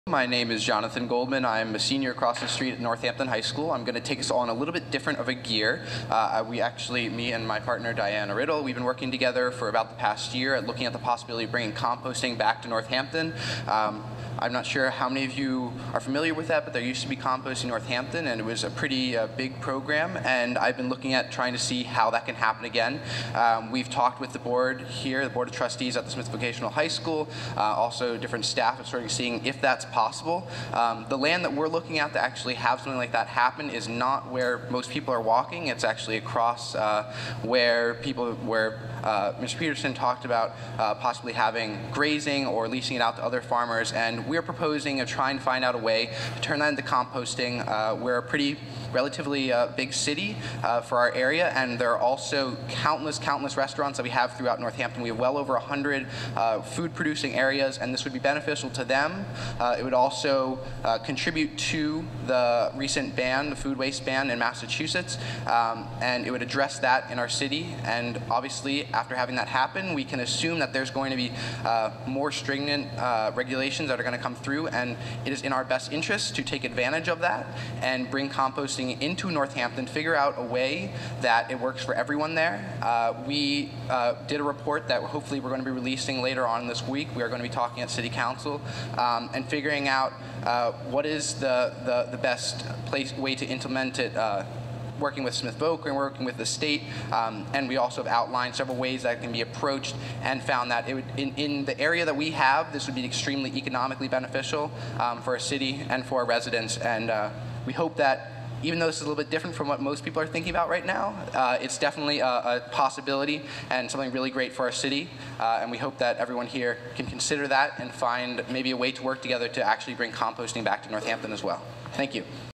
Northampton High student proposes composting station in town's unofficial "dog park."
Jonathan Goldman of Northampton High suggests creating a composting site on a portion of land owned by Smith Vocational. The 282-acre parcel was formerly the site of a state hospital. The land was set aside for agricultural use but has become an important recreational area in Northampton. The land lease is up for review and some city and state officials propose changing the way the land is used.